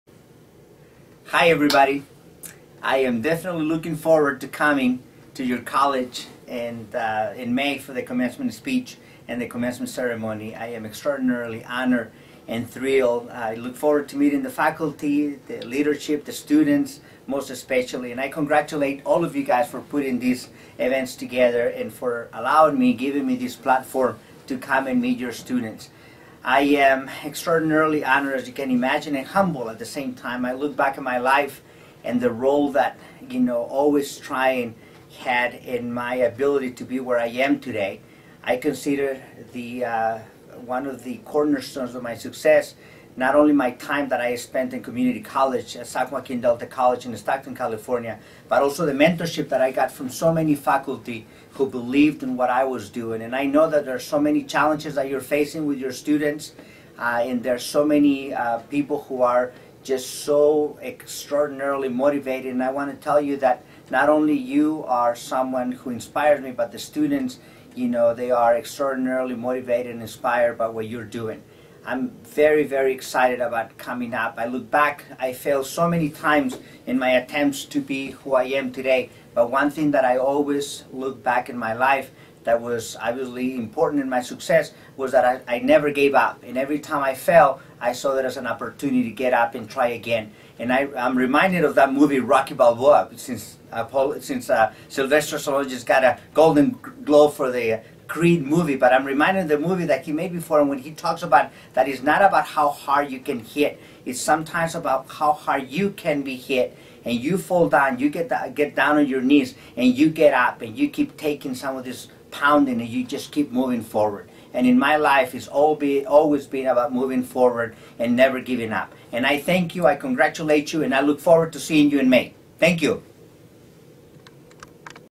Hello College of Marin! A Message from Dr. Q
California community college grad Dr. Alfredo Quiñones-Hinojosa "Dr. Q," is College of Marin's 2016 Commencement speaker. Dr. Q is looking forward to his visit in May, and we couldn't be more thrilled to hear his inspiring story of how community college transformed his life. He attributes his success on the journey from migrant farm worker to neurosurgeon to his experiences along the way, including his education at San Joaquin Delta College, and his never-give-up attitude.